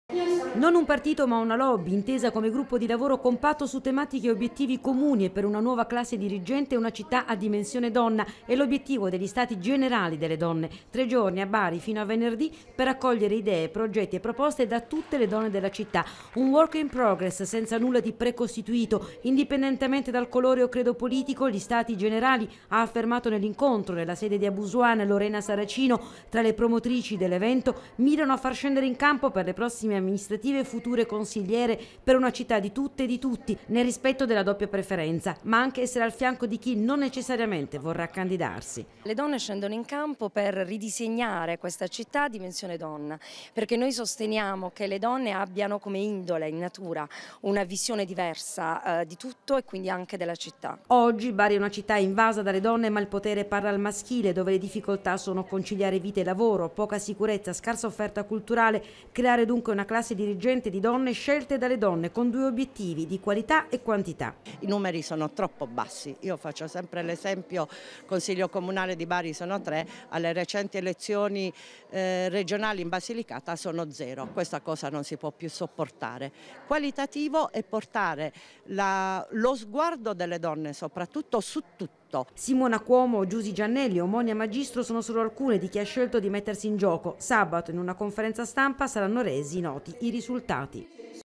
BARI STATI GENERALI DONNE PER NUOVA CLASSE DIRIGENTE
AL VIA A BARI LA TRE GIORNI DEGLI STATI GENERALI DELLE DONNE. OBIETTIVO: CREARE UNA NUOVA CLASSE DIRIGENTE E RACCOGLIERE CANDIDATE ALLE PROSSIME AMMINISTRATIVE. PER UNA CITTA' A DIMENSIONE DI DONNA